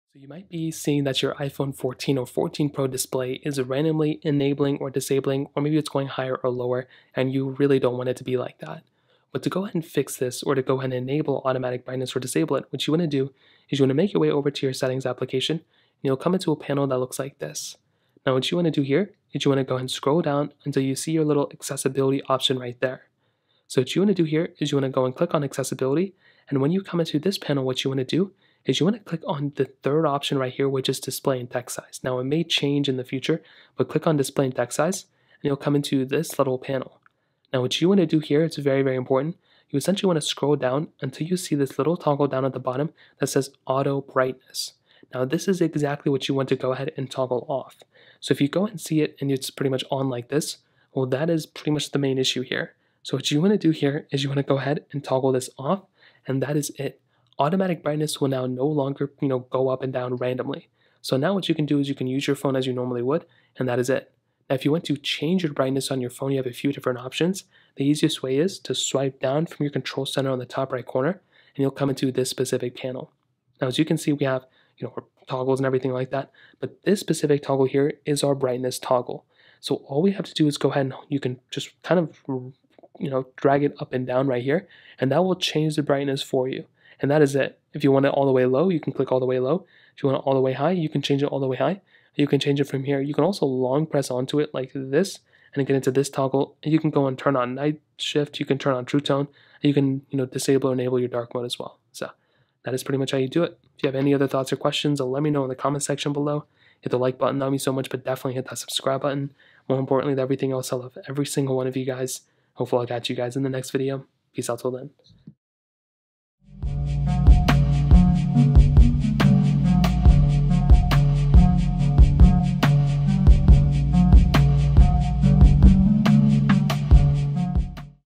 How To Turn On/Off Automatic Brightness On iPhone 14/iPhone 14 Pro!
Here is exactly How To Turn On/Off Automatic Brightness On iPhone 14/iPhone 14 Pro!